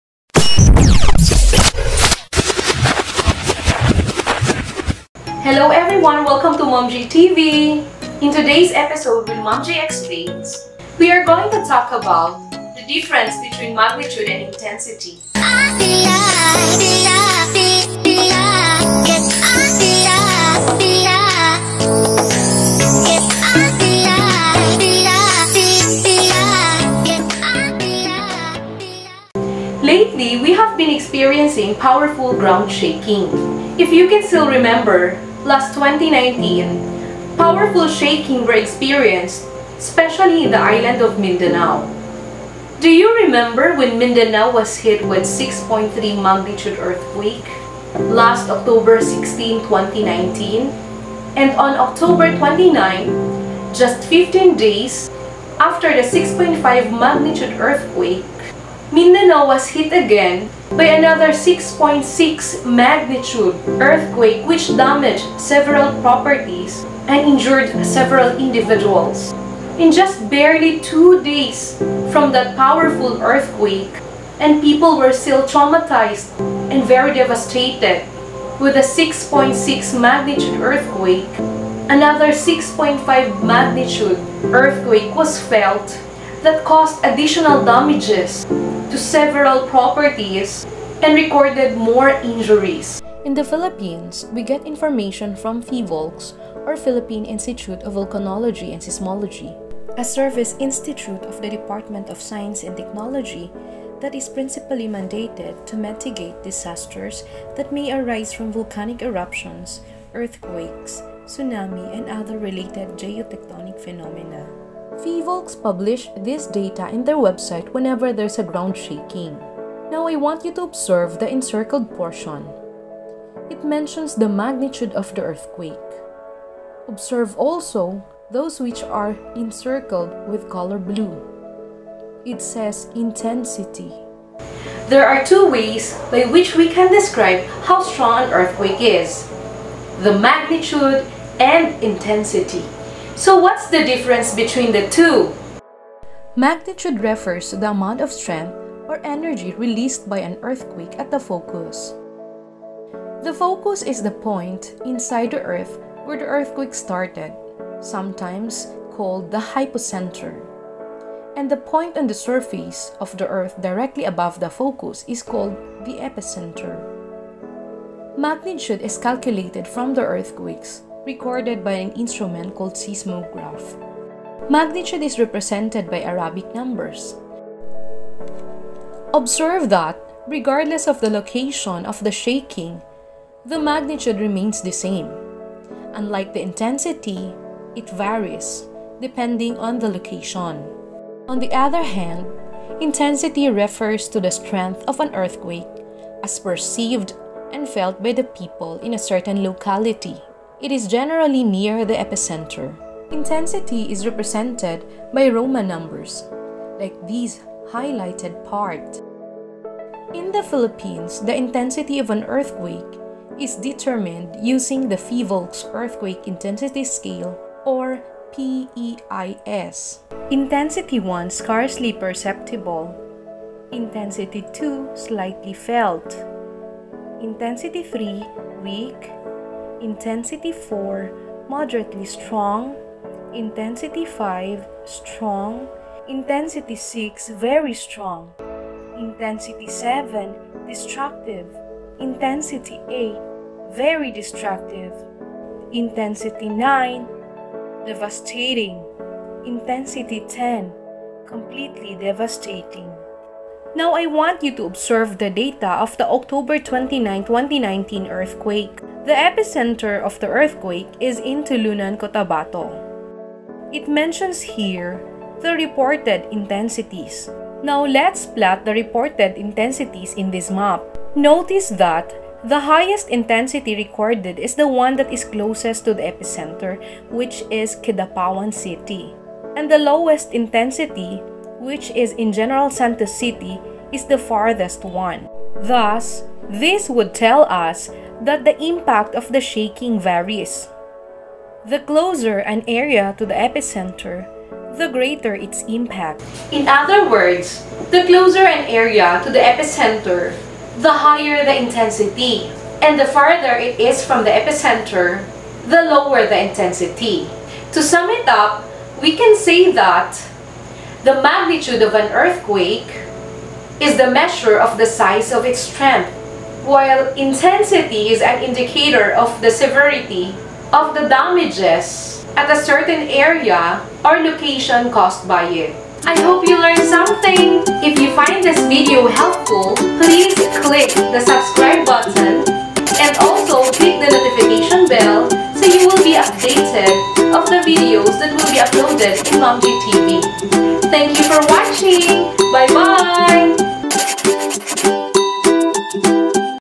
MAGNITUDE VS INTENSITY | Simplified Discussion | Maam Gie Explains | Episode 2
This video will talk about the difference between magnitude and intensity in the most simplified manner.